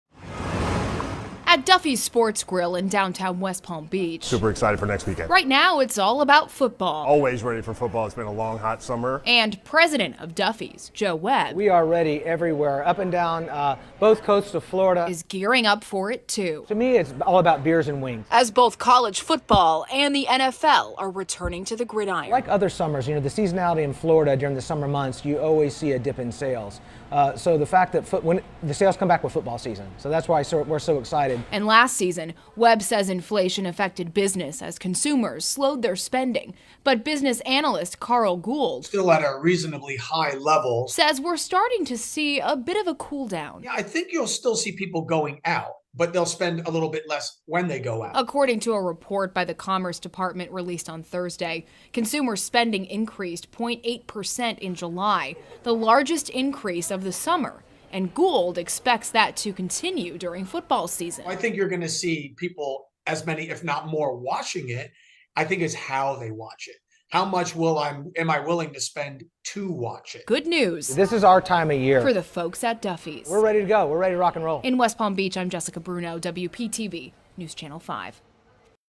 Businesses looks forward to NFL, college football boosting business
Football season is here. Whether its college or pro, fans are returning to stadiums and sports bars to cheer on their favorite teams.